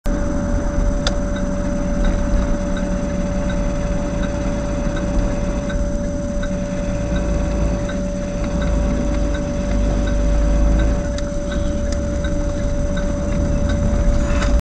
bicyclist
Bicyclist running stop sign. I wonder who would get blamed if a car hit him?